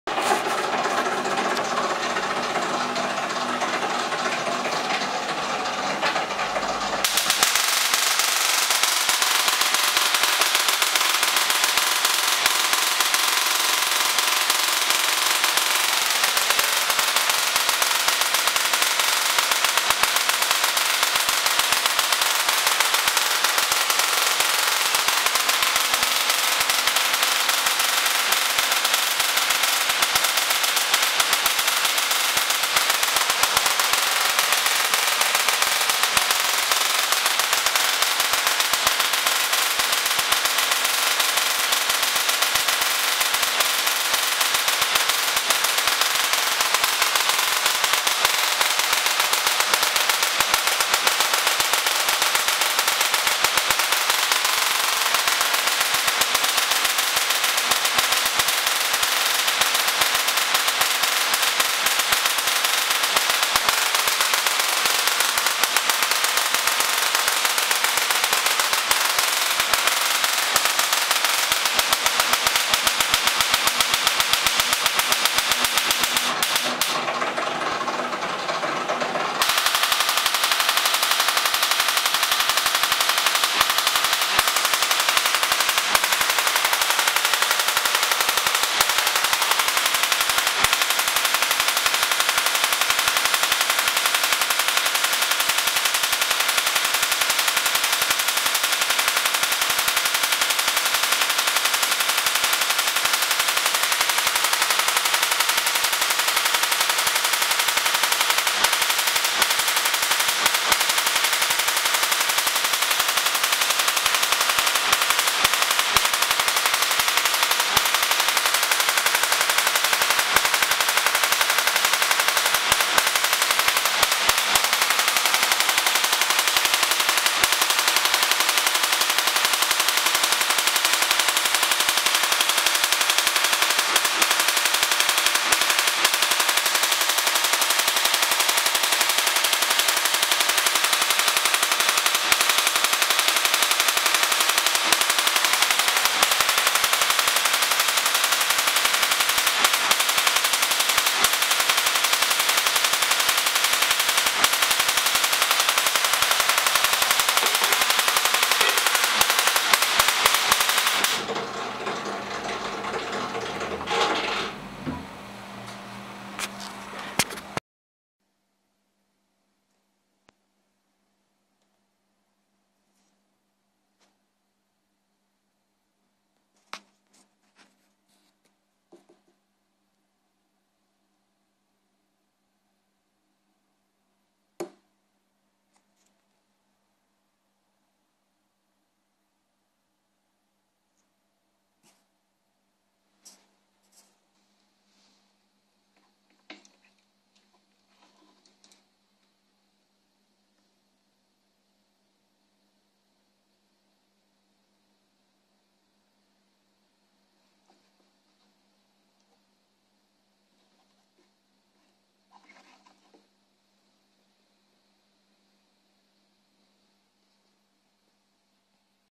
spark gap Tesla coil 100W
rotary spark gap Tesla coil
2e coil wireless coupled
TO220 mosfet(IRFB4332PBF) zvs flyback
+/- 100Watt input